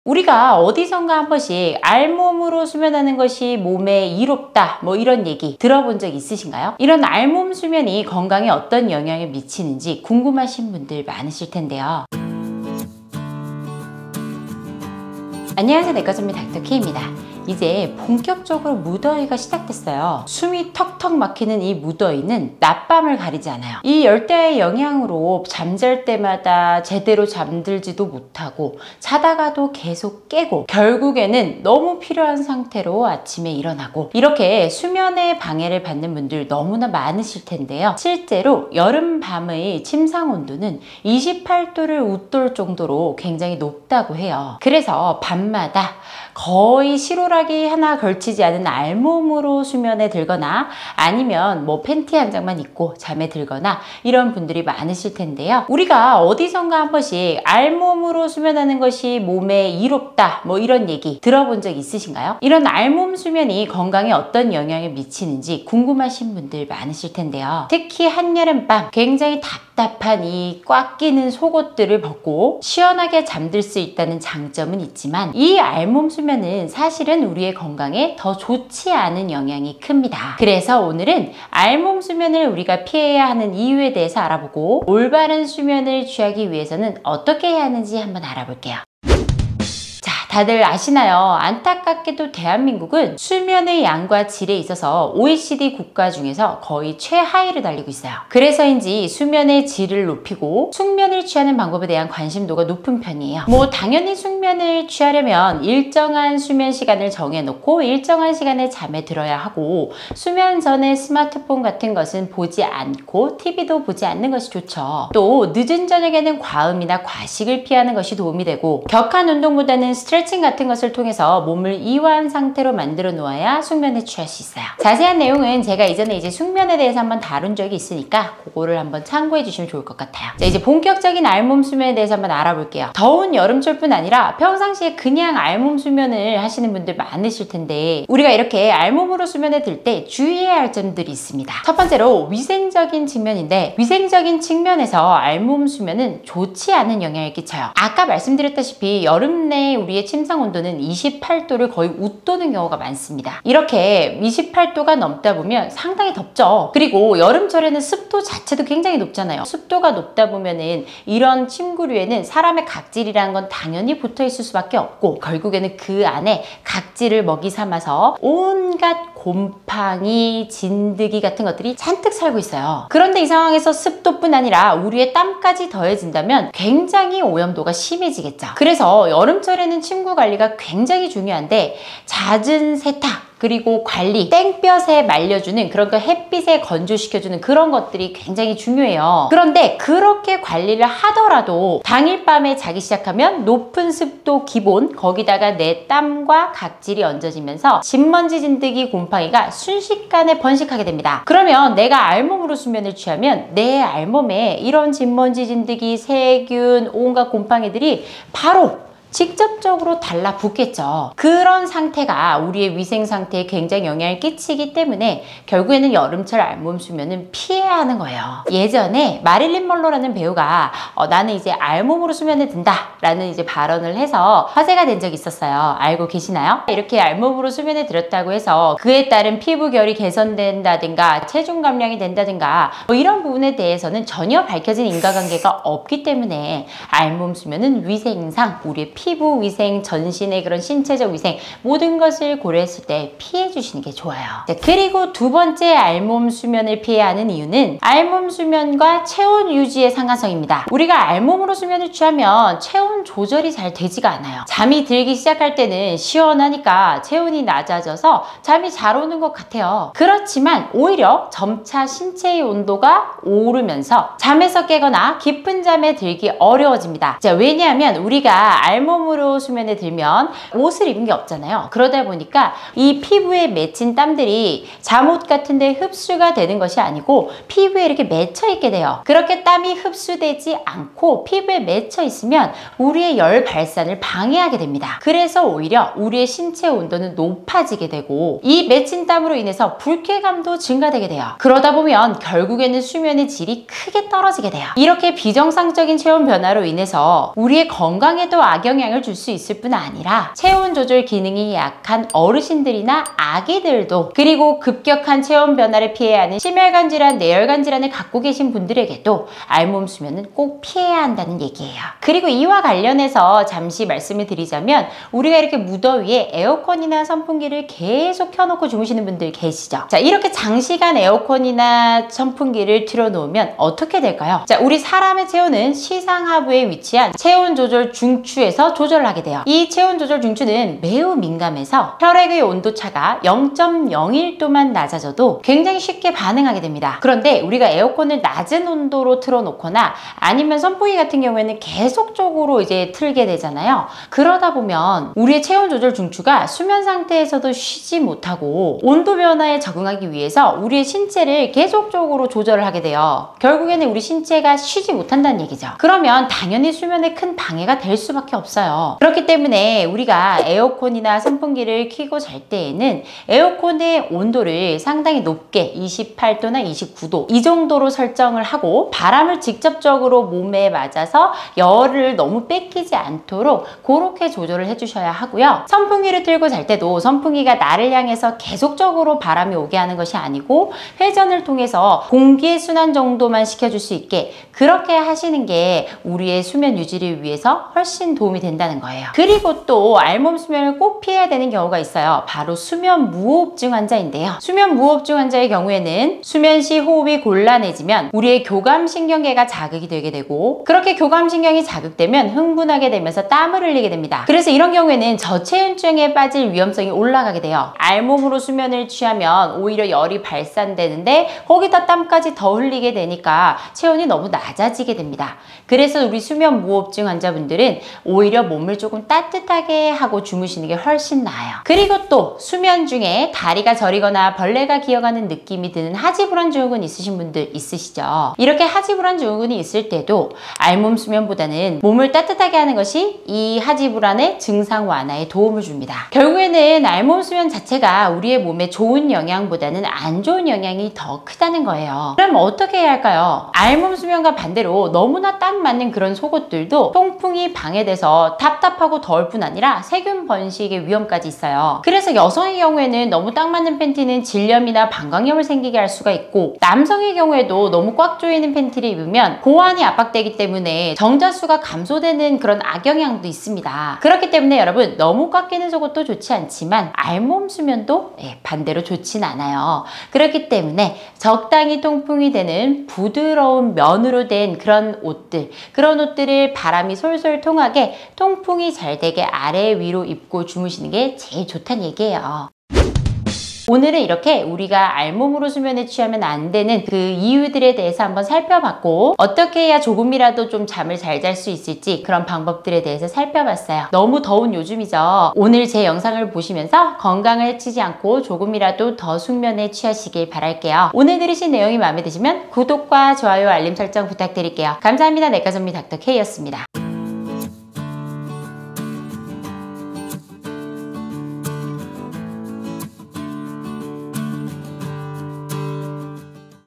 더운 여름, '알몸 수면'이 건강에 좋다는데 사실일까?
안녕하세요 내과 전문의 닥터케이입니다.
여름밤 침상 내 온도는 28도를 웃돌 정도로 매우 덥다고 합니다.
안 그래도 무더운 여름 날씨에 속옷과 옷을 입고 자는 게 갑갑하신 분들 많으실 텐데요.
어디선가 듣기로는 '알몸 수면이 건강에 이롭다고 했던 것 같은데?'
과연 이 말은 사실일까요?
오늘은 알몸 수면을 피해야 하는 이유에 대해서 알아보도록 하겠습니다.

▶내과전문의 닥터케이 비즈니스 관련 문의

00:00 하이라이트
01:29 숙면을 취하는 방법
02:21 '알몸 수면' 위생적인 측면에서 부적합
04:04 '알몸 수면' 체온 조절 측면에서 부적합
06:29 '알몸 수면' 수면 무호흡증 환자에게 부적합
07:22 속옷을 입고 자면?
08:03 정리